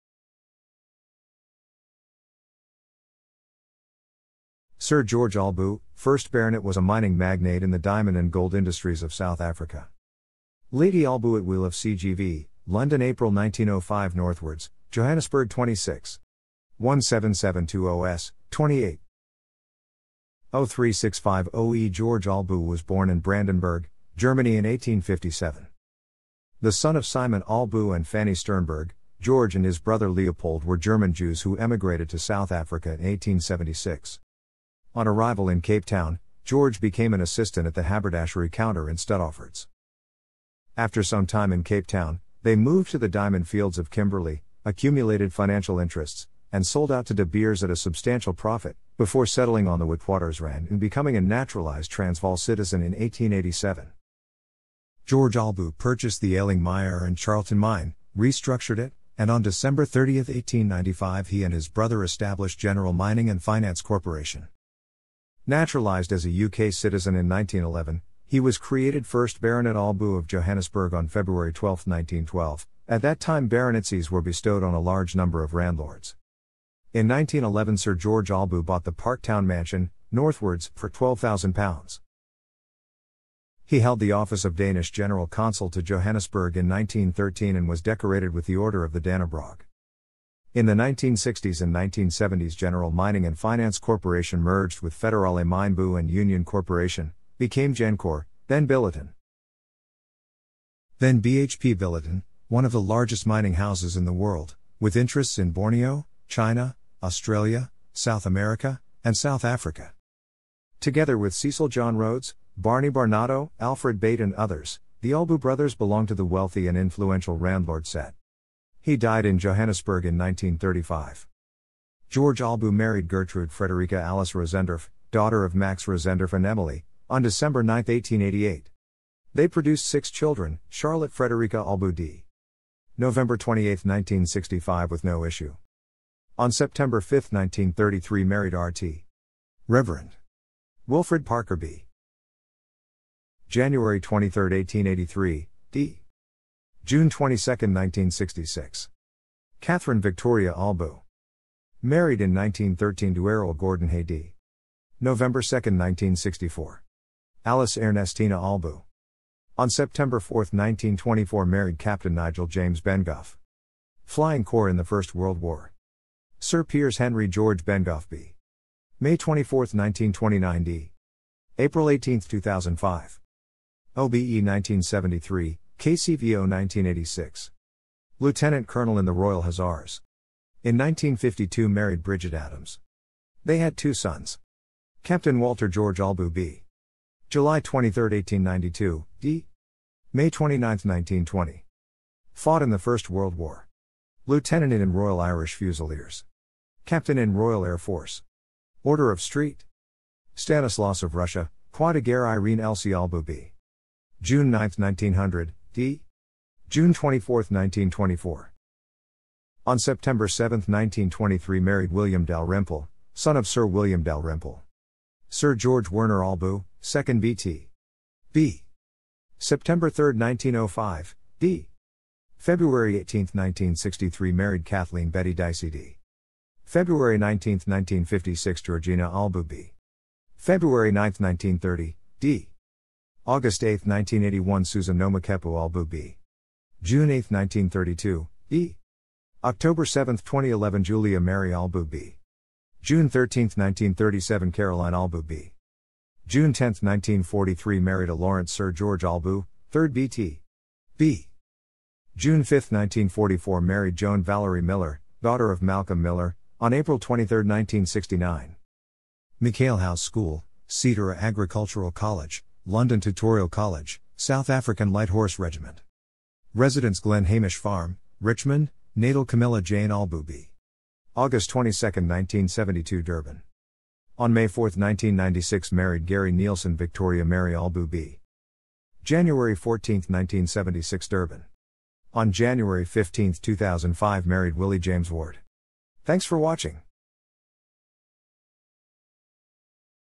George Albu
19th-century_German_Jews
Sir George Albu, 1st Baronet （26 October 1857 – 27 December 1935） was a mining magnate in the diamond and gold industries of South Africa.
Lady Albu at wheel of CGV, London April 1905 Northwards, Johannesburg 26.
17720S, 28.
03650E George Albu was born in Brandenburg, Germany in 1857.
The son of Simon Albu （26 February 1830 – 26 February 1911） and Fanny Sternberg （d. 24 October 1912）, George and his brother Leopold were German Jews who emigrated to South Africa in 1876.
On arrival in Cape Town, George became an assistant at the haberdashery counter in Stuttafords.
After some time in Cape Town, they moved to the diamond-fields of Kimberley, accumulated financial interests, and sold out to De Beers at a substantial profit, before settling on the Witwatersrand and becoming a naturalized Transvaal citizen in 1887.
George Albu purchased the ailing Meyer and Charlton Mine, restructured it, and on 30 December 1895 he and his brother established General Mining and Finance Corporation.
Naturalized as a UK citizen in 1911, he was created 1st Baronet Albu of Johannesburg on 12 February 1912 — at that time baronetcies were bestowed on a large number of Randlords.
In 1911 Sir George Albu bought the Parktown mansion, Northwards, （designed by Herbert Baker in 1904） for £12,000 （Colonel John Dale Lace and wife Josephine, the previous owners, had paid £21 000 for the house）.
He held the office of Danish General Consul to Johannesburg in 1913 and was decorated with the Order of the Dannebrog.
In the 1960s and 1970s General Mining and Finance Corporation merged with Federale Mynbou and Union Corporation, became Gencor, then Billiton,
then BHP Billiton, one of the largest mining houses in the world, with interests in Bo...